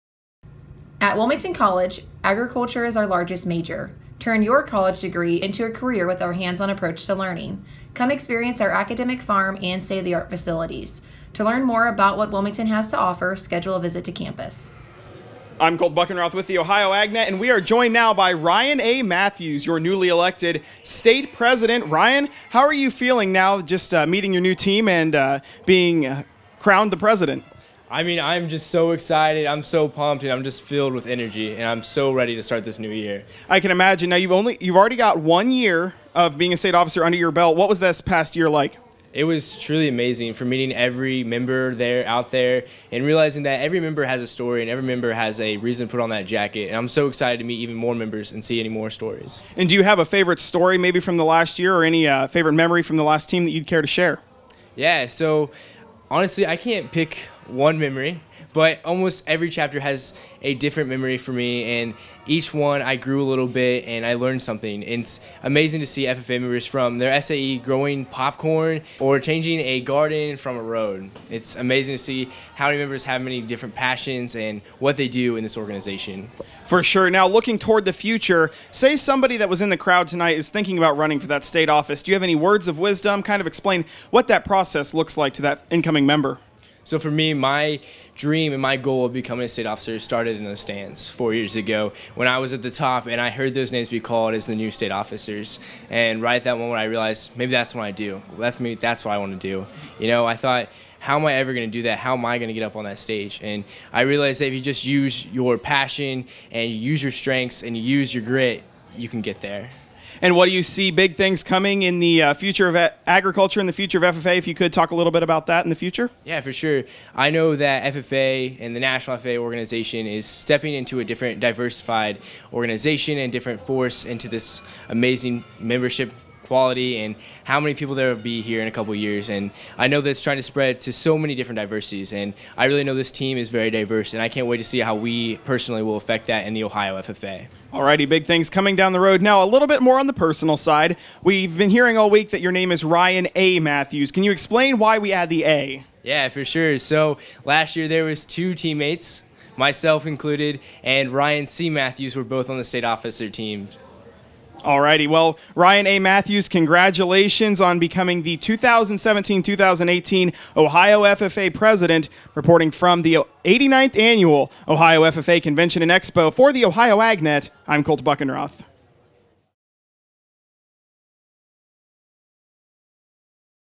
Introducing Ryan Matthews, 2017-2018 Ohio FFA President
Student FFA Reporter Kolt Buchenroth interviewed Ryan A. Matthews shortly after he was named the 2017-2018 Ohio FFA President.